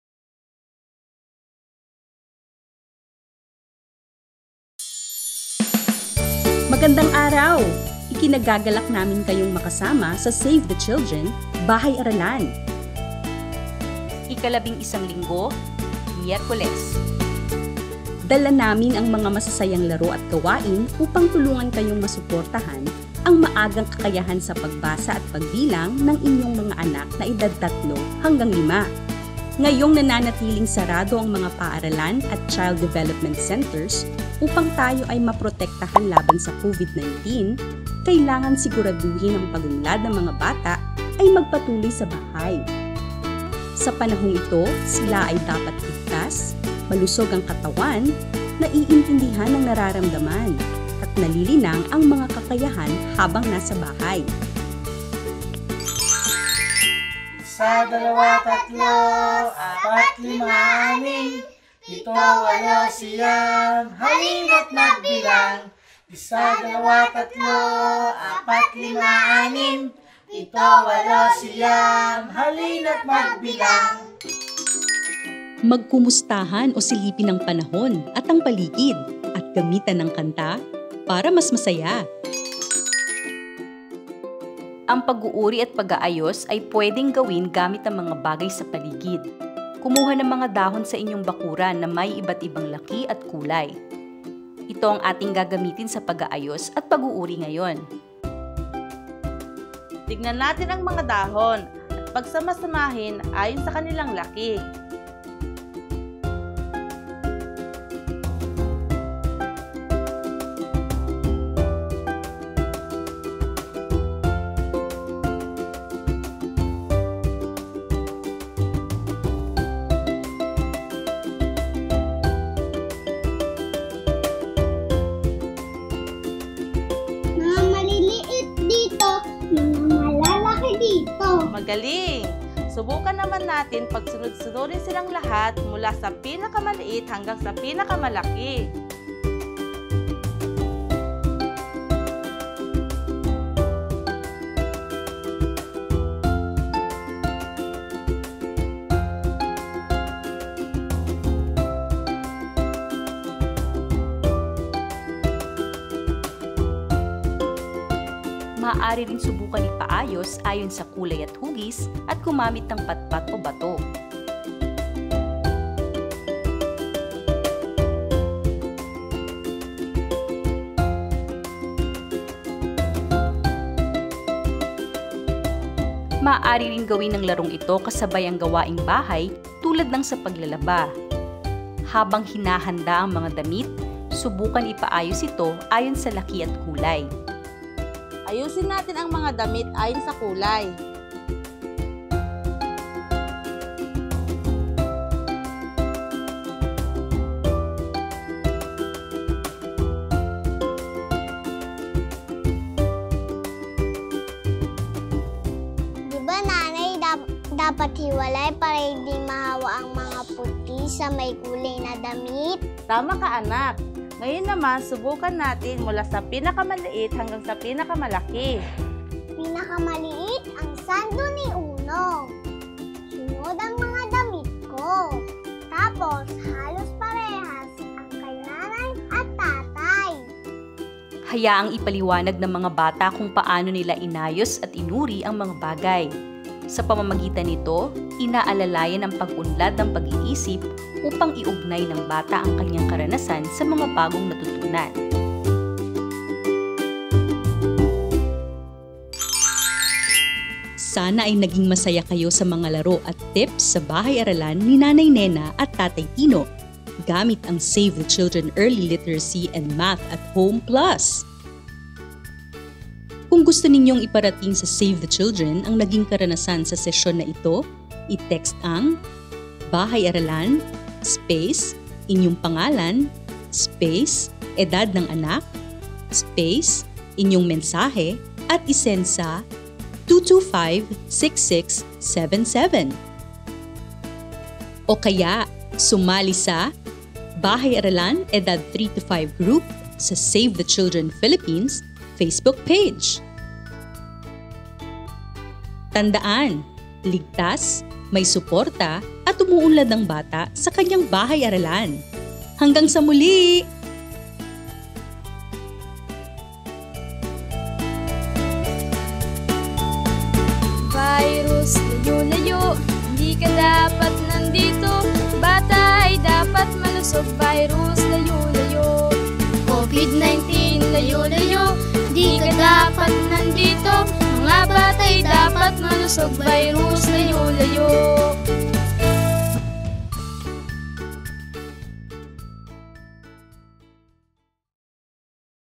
BahayAral: Edad 3-5 (Linggo 11, Miyerkules)
Ang pag-uuri at pag-aayos ay pwede gawin gamit ang mga bagay sa paligid. Isang halimbawa ay ang mga dahon na mapupulot sa inyong bakuran. Ito ang gagamitin natin ngayon sa BahayAralan para sa ating pag-uuri at pag-aayos kaya siguraduhing mamili ng mga dahon na magkakaiba ng laki at kulay.

Ang BahayAralan para sa mga batang edad 3-5 ay mga maiikling sesyon ng masasayang laro at gawain na tatagal ng 12 linggo, gamit ang aming Early Literacy and Math (ELM) at Home Plus+ modules.